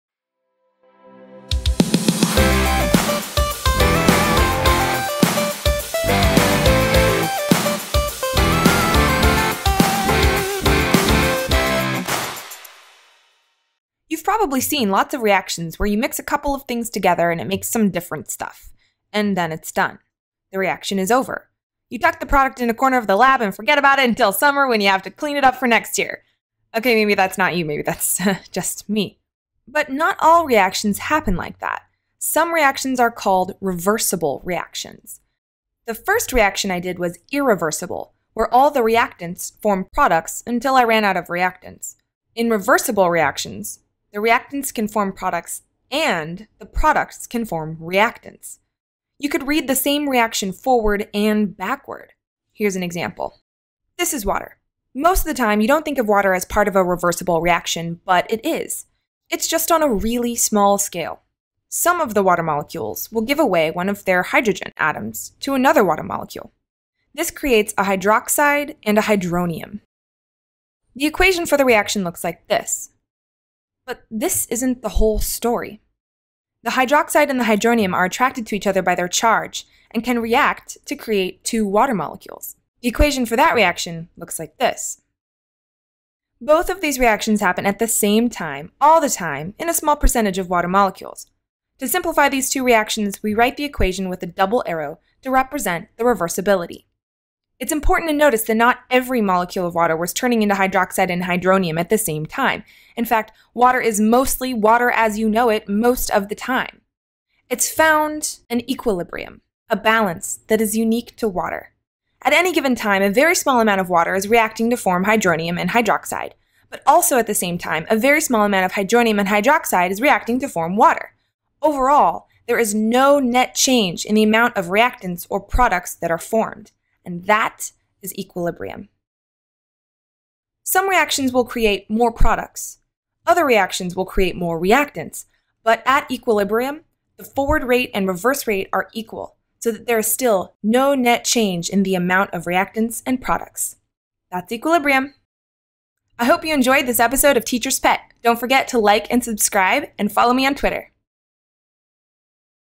Intro to Equilibrium
Learn the basics of chemical equilibrium using the dissociation of water as an example.
Intro 0:14
Reversible vs irreversible 0:29
Dissociation of water 0:53
Definition of equilibrium 1:52